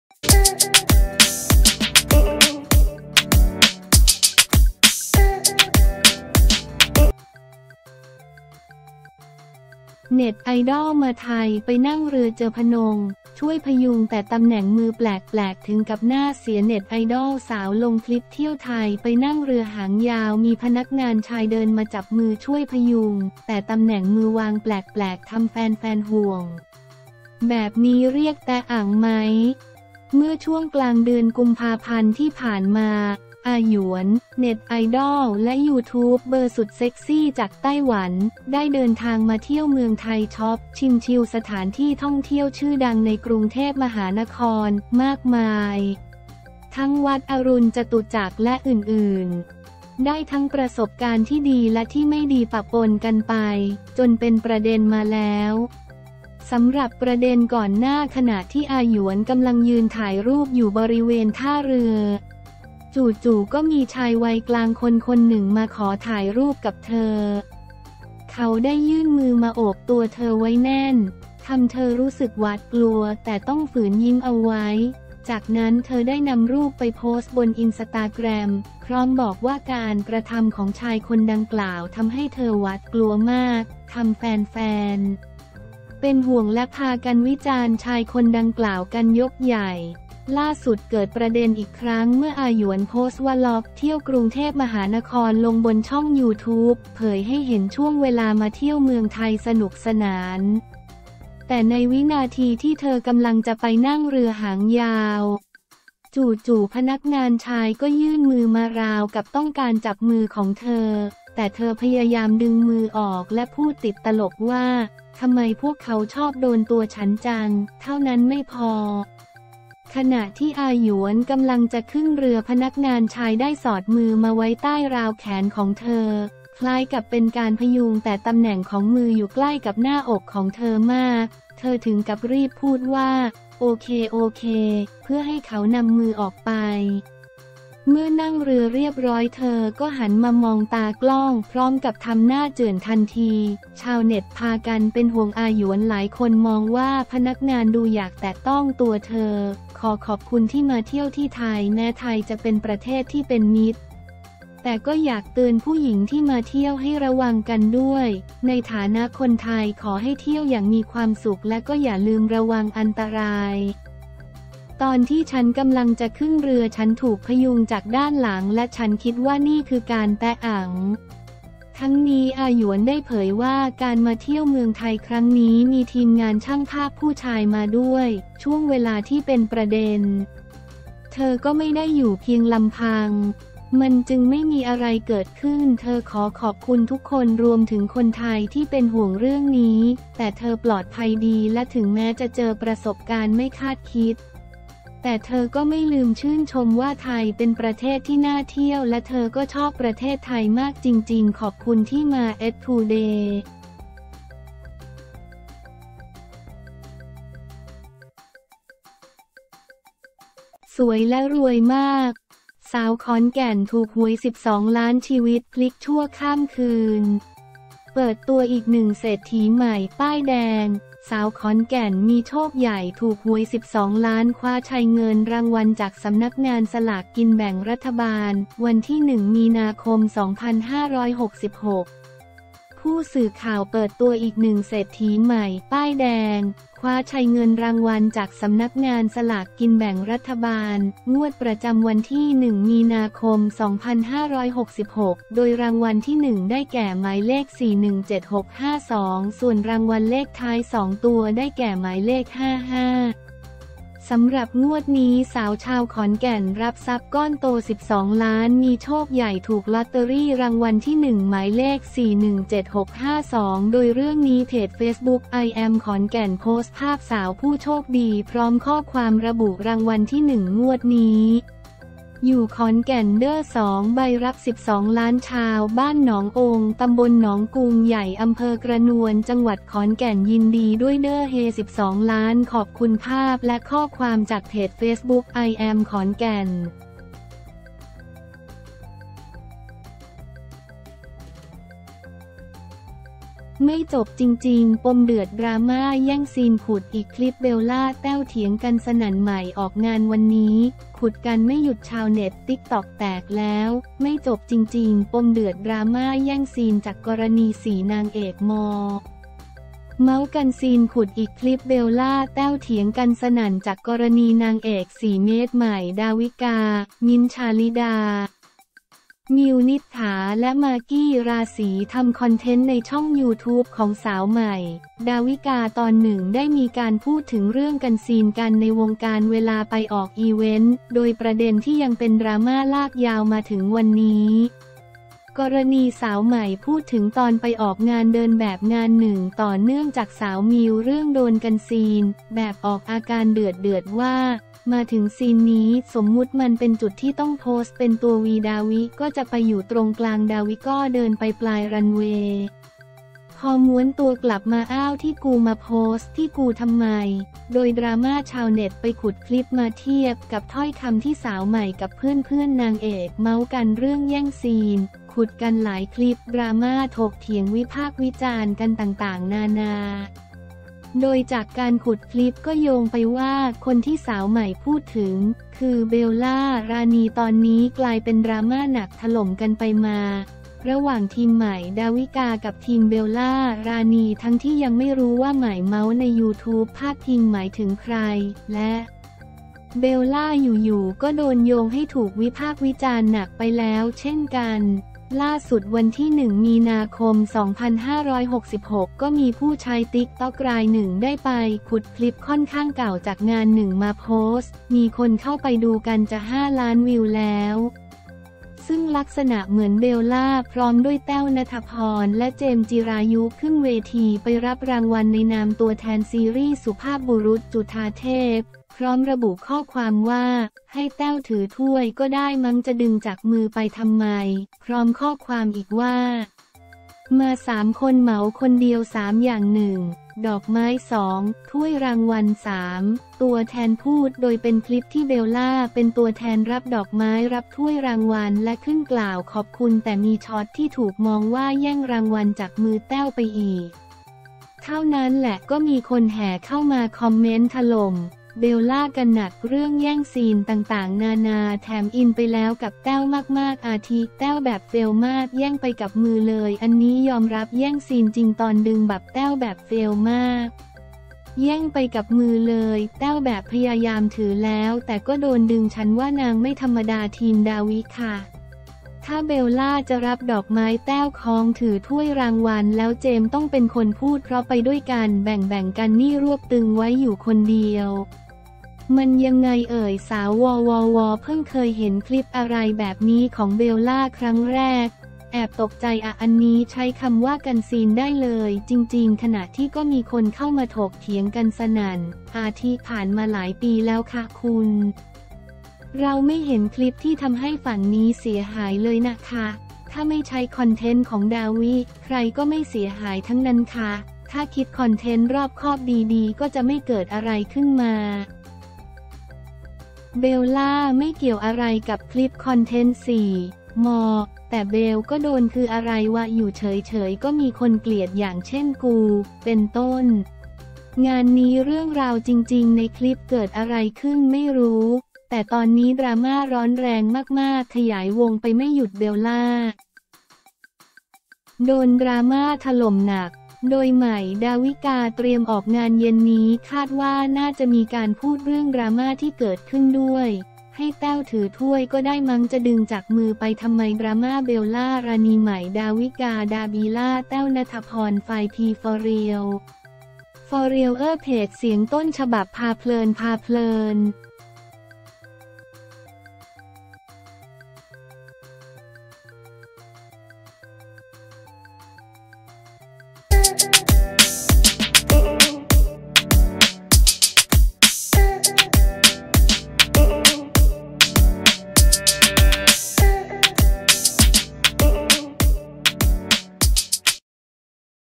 เน็ตไอดอลมาไทย ไปนั่งเรือ เจอพนง ช่วยพยุง แต่ตำแหน่งมือแปลกๆ ถึงกับหน้าเสีย
เน็ตไอดอลสาวลงคลิปเที่ยวไทย ไปนั่งเรือหางยาว มีพนักงานชายเดินมาจับมือ-ช่วยพยุง แต่ตำแหน่งมือวางแปลก ๆ ทำแฟน ๆ ห่วง แบบนี้เรียกแต๊ะอั๋งมั้ย?

เมื่อช่วงกลางเดือนกุมภาพันธ์ที่ผ่านมา “อาหยวน” เน็ตไอดอลและยูทูบเบอร์สุดเซ็กซี่จากไต้หวัน ได้เดินทางมาเที่ยวเมืองไทย ช็อป ชิม ชิล สถานที่ท่องเที่ยวชื่อดังในกรุงเทพฯ มากมาย ทั้งวัดอรุณฯ จตุจักร ฯลฯ ได้ทั้งประสบการณ์ที่ดีและที่ไม่ดีปะปนกันไป จนเป็นประเด็นมาแล้ว

สำหรับประเด็นก่อนหน้า ขณะที่อาหยวนกำลังยืนถ่ายรูปอยู่บริเวณท่าเรือ จู่ ๆ ก็มีชายวัยกลางคนคนหนึ่งมาขอถ่ายรูปกับเธอ เขาได้ยื่นมือมาโอบตัวเธอไว้แน่น ทำเธอรู้สึกหวาดกลัวแต่ต้องฝืนยิ้มเอาไว้ จากนั้นเธอได้นำรูปไปโพสต์บนอินสตาแกรม พร้อมบอกว่าการกระทำของชายคนดังกล่าวทำให้เธอหวาดกลัวมาก ทำแฟน ๆ เป็นห่วงและพากันวิจารณ์ชายคนดังกล่าวกันยกใหญ่
ล่าสุด เกิดประเด็นอีกครั้ง เมื่ออาหยวนโพสต์วล็อกเที่ยวกรุงเทพฯลงบนช่องยูทูบ เผยให้เห็นช่วงเวลามาเที่ยวเมืองไทยสนุกสนาน แต่ในวินาทีที่เธอกำลังจะไปนั่งเรือหางยาว จู่ ๆ พนักงานชายก็ยื่นมือมาราวกับต้องการจับมือของเธอ แต่เธอพยายามดึงมือออก และพูดติดตลกว่า “ทำไมพวกเขาชอบโดนตัวฉันจัง”

เท่านั้นไม่พอ ขณะที่อาหยวนกำลังจะขึ้นเรือ พนักงานชายได้สอดมือมาไว้ใต้ราวแขนของเธอ คล้ายกับเป็นการพยุง แต่ตำแหน่งของมืออยู่ใกล้กับหน้าอกของเธอมาก เธอถึงกับรีบพูดว่า “โอเค ๆ” เพื่อให้เขานำมือออกไป เมื่อนั่งเรือเรียบร้อย เธอก็หันมามองตากล้องพร้อมกับทำหน้าเจื่อนทันที

ชาวเน็ตพากันเป็นห่วงอาหยวน หลายคนมองว่าพนักงานดูอยากแตะต้องตัวเธอ “ขอขอบคุณที่มาเที่ยวที่ไทย แม้ไทยจะเป็นประเทศที่เป็นมิตร แต่ก็อยากเตือนผู้หญิงที่มาเที่ยวให้ระวังกันด้วย ในฐานะคนไทยขอให้เที่ยวอย่างมีความสุขและก็อย่าลืมระวังอันตราย”, “ตอนที่ฉันกำลังจะขึ้นเรือ ฉันถูกพยุงจากด้านหลังและฉันคิดว่านี่คือการแต๊ะอั๋ง
ทั้งนี้ อาหยวนได้เผยว่า การมาเที่ยวเมืองไทยครั้งนี้มีทีมงานช่างภาพผู้ชายมาด้วย ช่วงเวลาที่เป็นประเด็น เธอก็ไม่ได้อยู่เพียงลำพัง มันจึงไม่มีอะไรเกิดขึ้น เธอขอขอบคุณทุกคน รวมถึงคนไทยที่เป็นห่วงเรื่องนี้ แต่เธอปลอดภัยดี และถึงแม้จะเจอประสบการณ์ไม่คาดคิด แต่เธอก็ไม่ลืมชื่นชมว่าไทยเป็นประเทศที่น่าเที่ยวและเธอก็ชอบประเทศไทยมากจริง ๆ
ขอบคุณที่มา ettoday